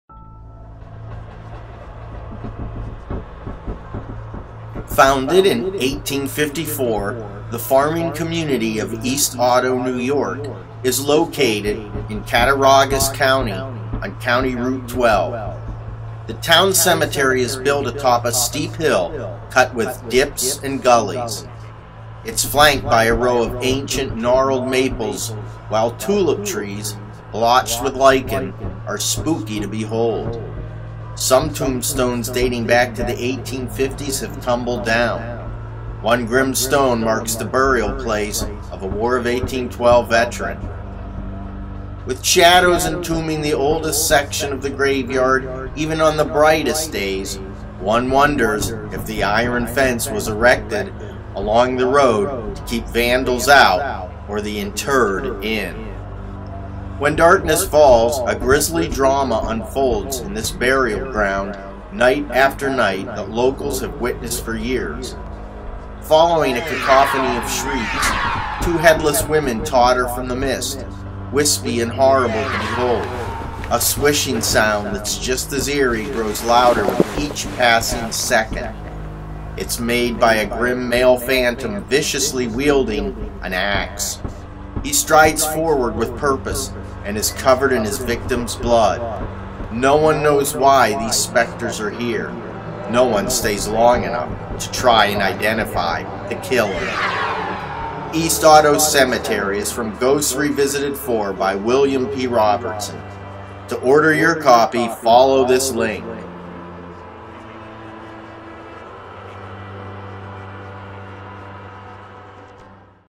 East Otto Cemetery
East Otto Cemetery in Cattaraugus County New York is home to a disturbing haunting that recurs night after night. It begins with women's shrieks and the menacing swish of an ax!